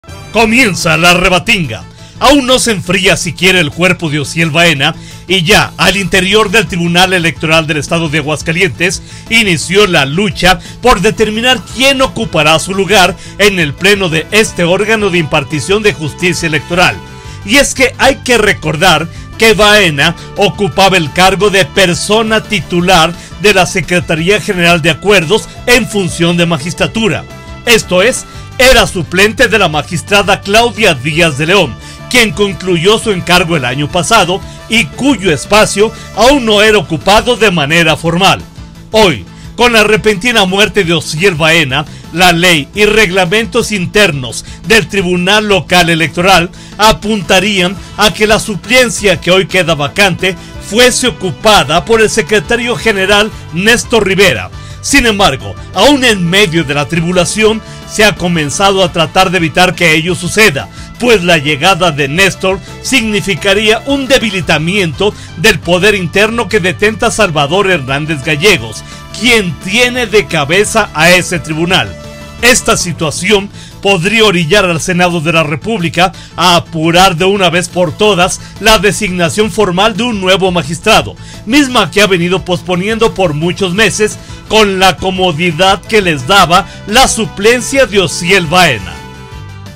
Crónica del Poder/Comienza la rebatinga
Comienza la rebatinga

Aguascalientes, Ags. 15 noviembre 2023.-  Aún no se enfría el cuerpo de Ociel Bahena y ya al interior del Tribunal Electoral del Estado de Aguascalientes inició la lucha por determinar quien ocupará su lugar en el pleno de este órgano de impartición de justicia electoral.
Y es que hay que recordar que Bahena ocupaba el cargo de persona titular de la Secretaría General de Acuerdos en función de magistratura, esto es, era suplente de la magistrada Claudia Díaz de León, quien concluyó su encargo el año pasado y cuyo espacio aún no era ocupado de manera formal.
Hoy, con la repentina muerte de Ociel Bahena, la ley y reglamentos internos del Tribunal Local Electoral apuntarían a que la suplencia que hoy queda vacante fuese ocupada por el Secretario General Nestor Rivera, sin embargo aún en medio de la tribulación, se ha comenzado a tratar de evitar que ello suceda, pues la llegada de Nestor significaría un debilitamiento del poder interno que detenta Salvador Hernández Gallegos, quien tiene de cabeza al Tribunal.
Esta situación podría orillar al Senado de la República a apurar la designación formal de un nuevo magistrado, misma que ha venido posponiendo por muchos meses con la comodidad que les daba la suplencia de Ociel Bahena.